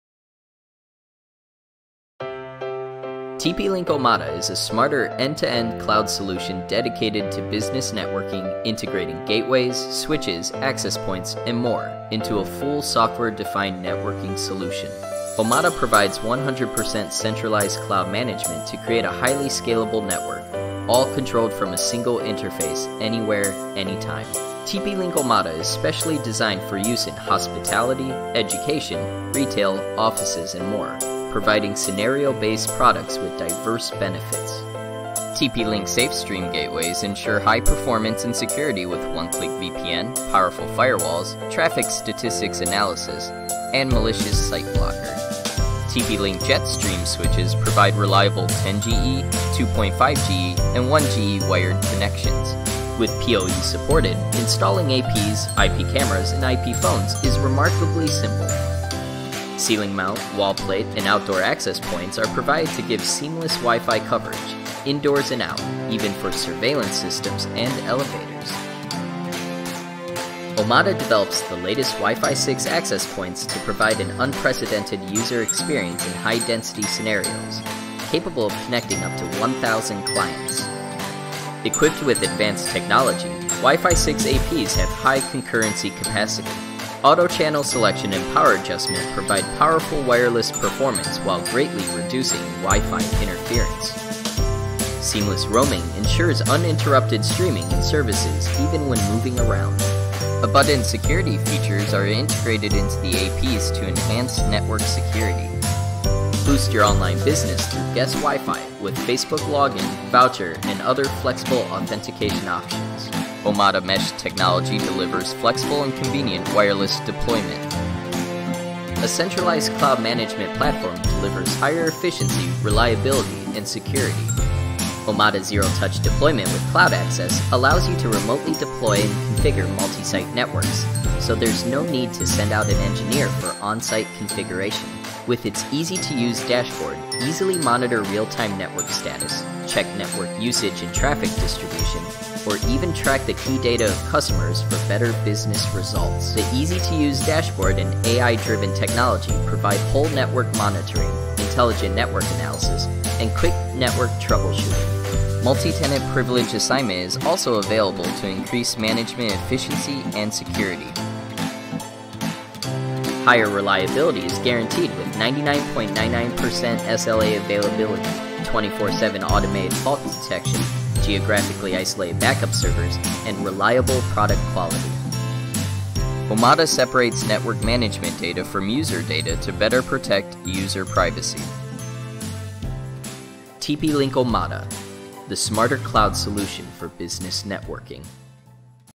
Soluciones Wi-Fi Empresariales: Omada SDN Cloud Wi-Fi 6
La gestión Centralizada Gratuita Coge el Control de Centenas de Dispositivos Omada a la Vez
Omada permite a los administradores de red la monitorización y gestión de todos los productos Omada en la red gracias a la plataforma de gestión centralizada. Esto permite un increíble escalado inalámbrico, ahorrando tiempo y sin la necesidad de personal IT .
Para consultas técnicas contactar con nuestro soporte enterprise, en español: chat